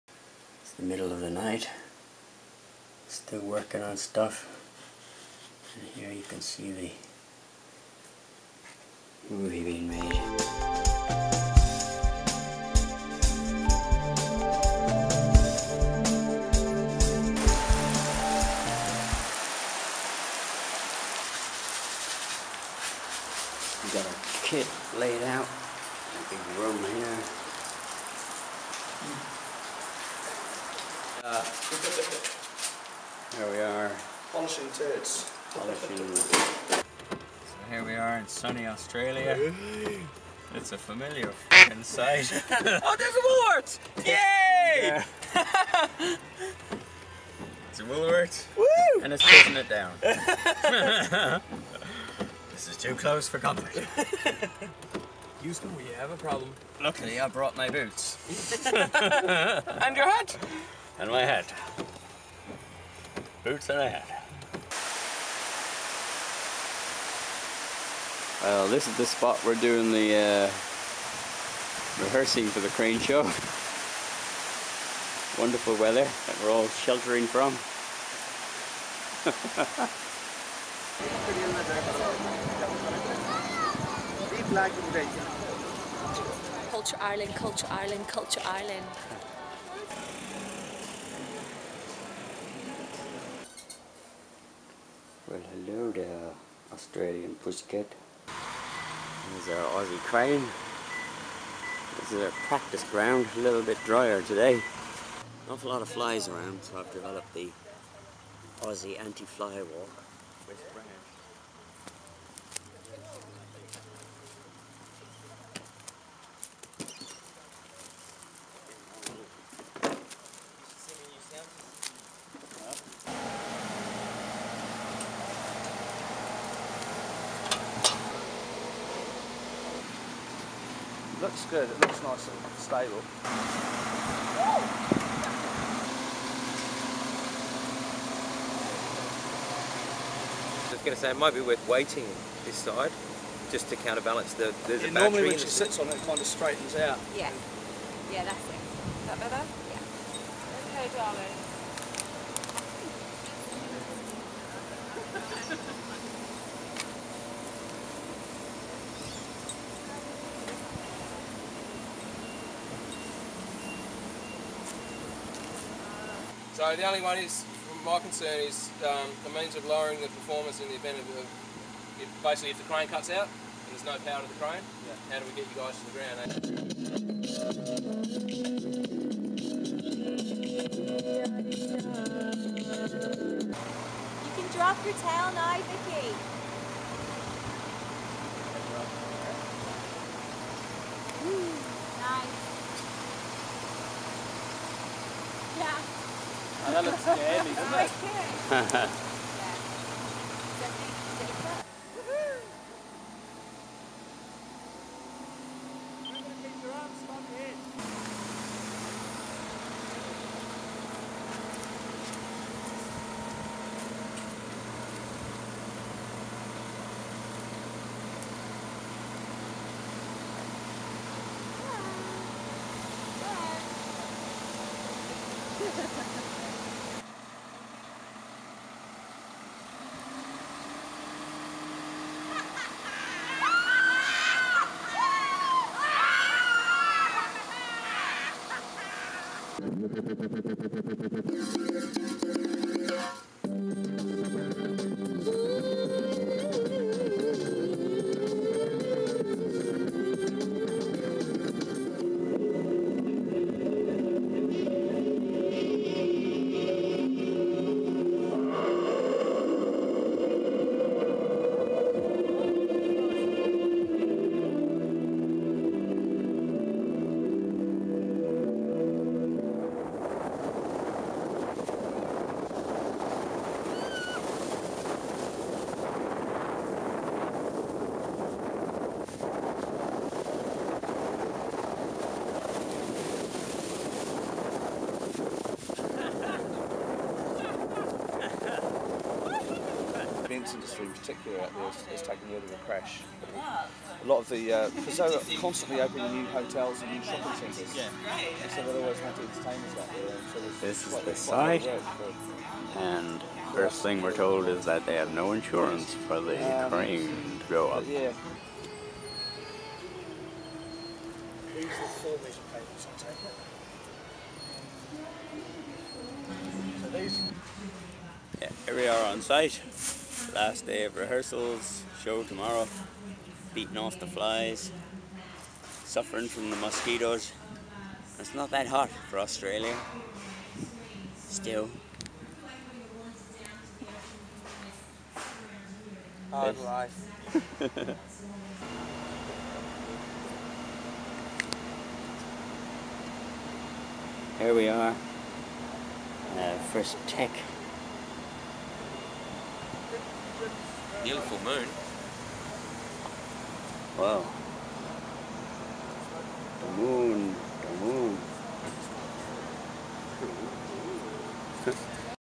The making of W&F in Oz
Some of Jyms 'video diary' of fidget feets trip to Austrailia where they put on their crane show.